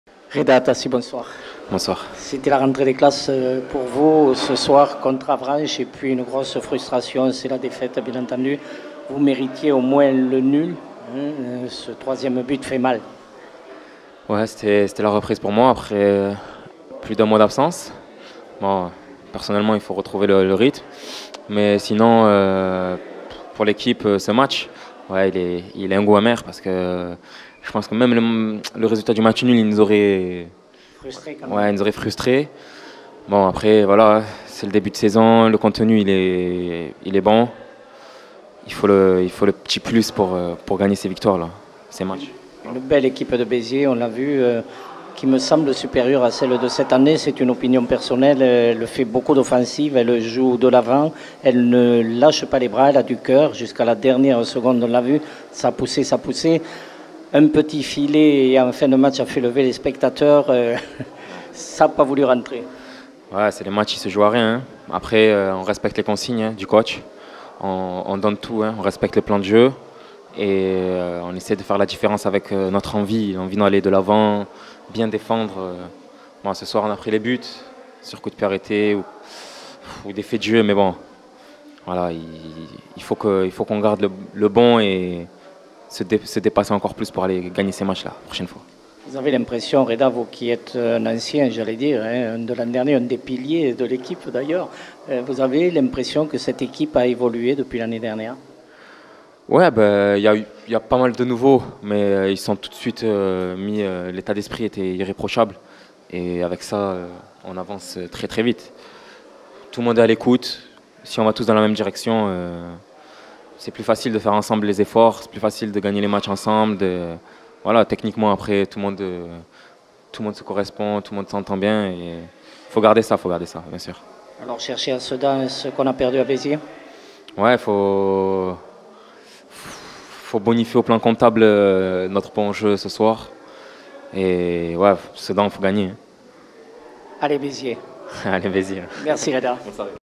FOOTBALL - Défaite contre Avranches : Rédah ATASSI
Interview du milieu de terrain de l'ASB Rédah Atassi suite à la défaite de Béziers contre Avranches à domicile (2-3)